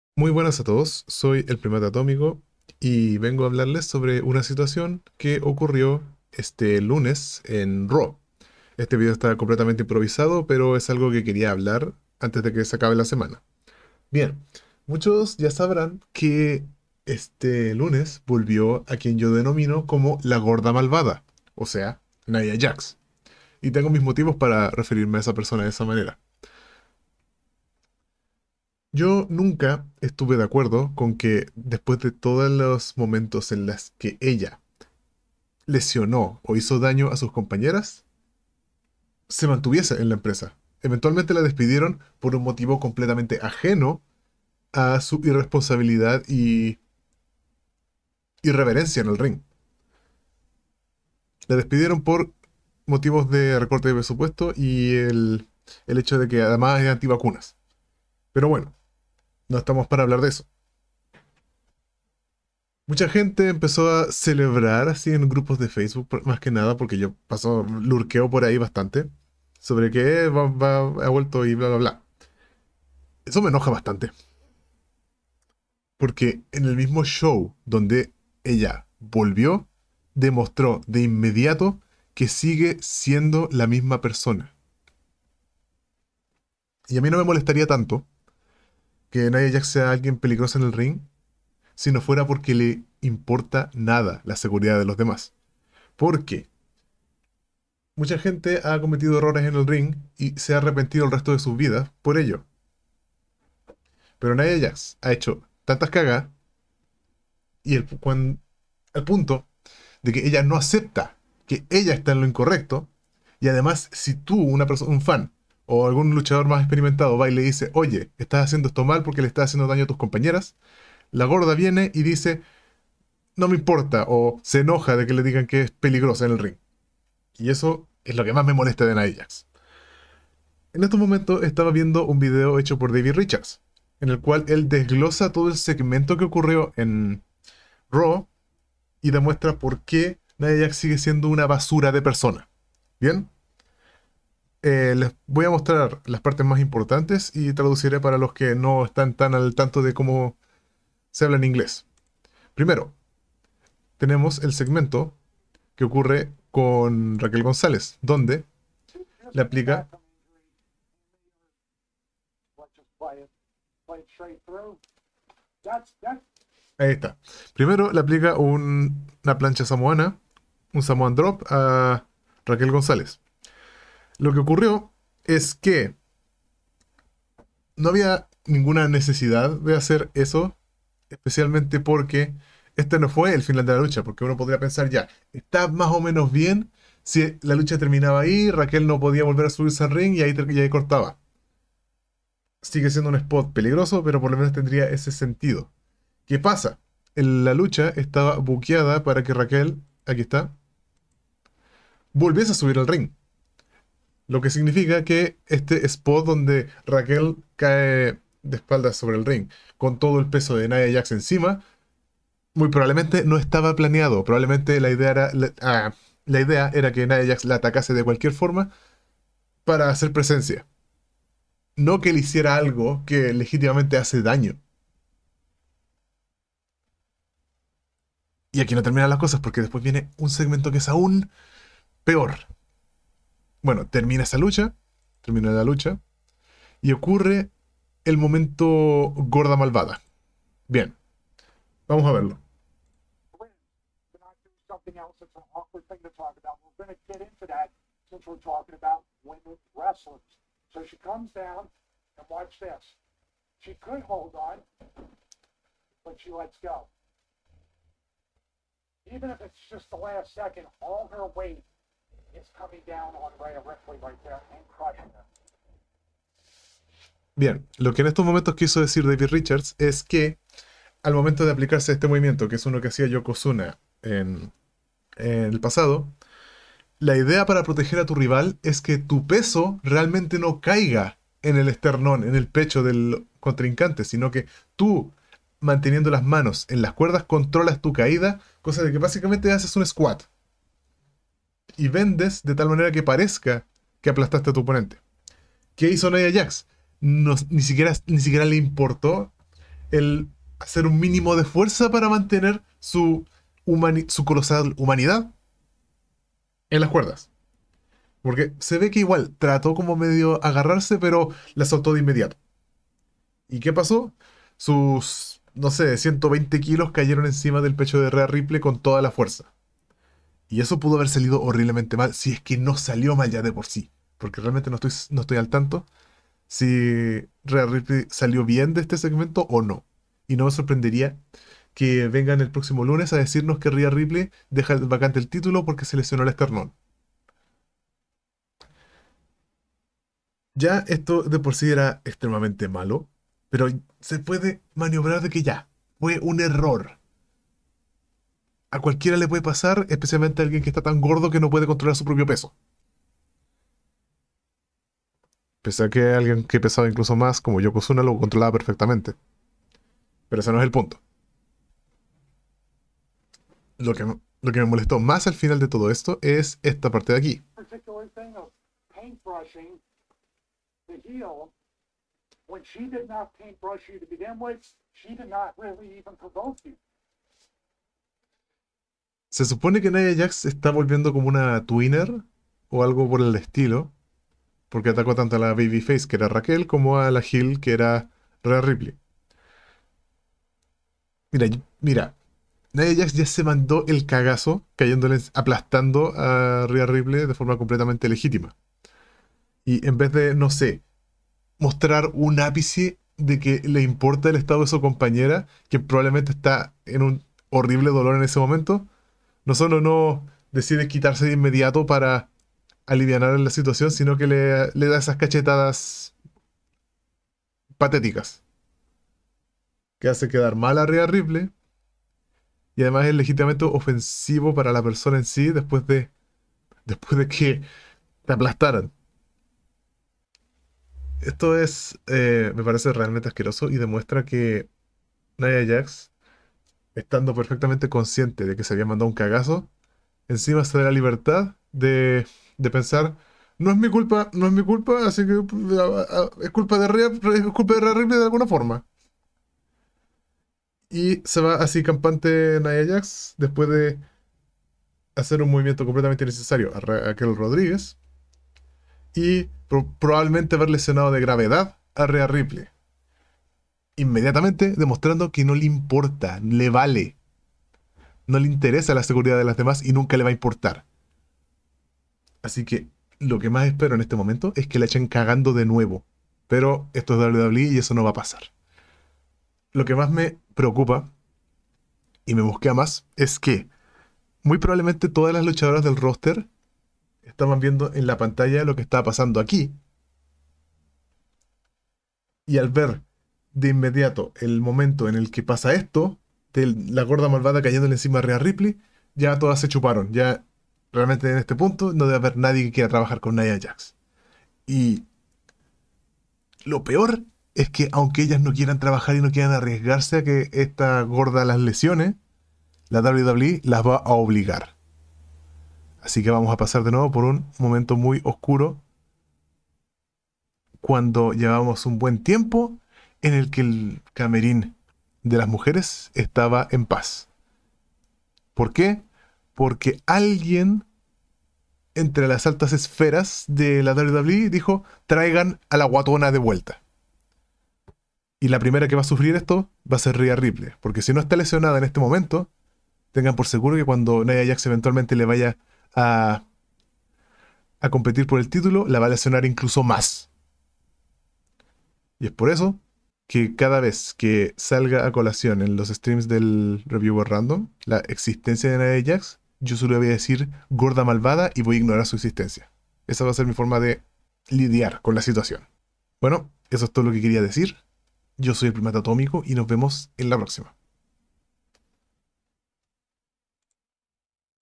Nia Jax no ha cambiado y nunca lo hara
Mi opinión de lo ocurrido en Raw de esta semana.